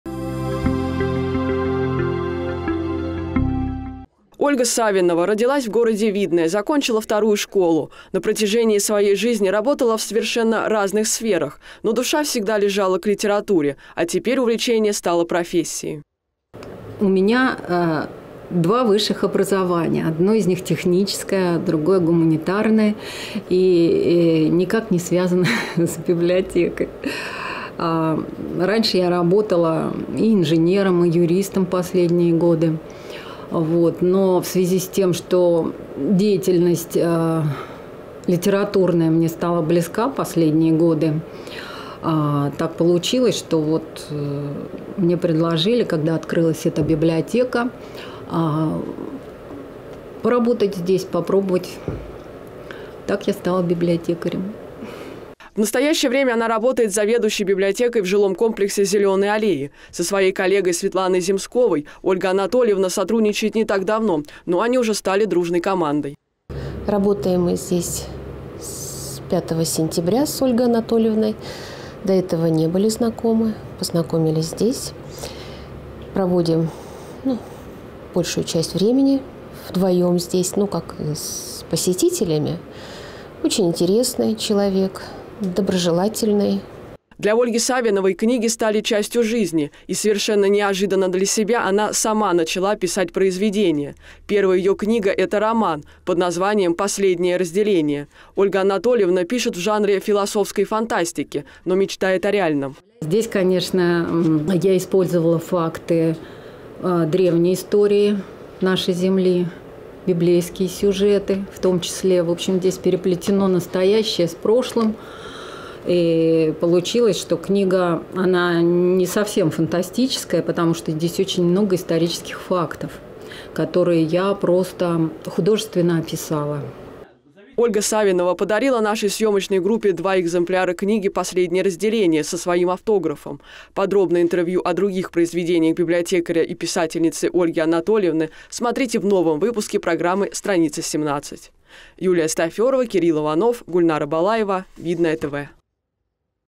В МИРЕ КНИГ
В Ленинском округе живут и работают много интересных и талантливых людей. Наш корреспондент Юлия Стаферова расскажет о жизни и о творчестве коренной видовчанки, библиотекаре и писательнице Ольге Савиновой.